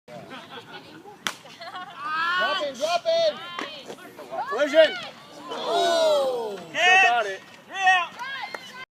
Softball, Outfielders Running into each other, Awesome Video
Softball, Outfielders running into each other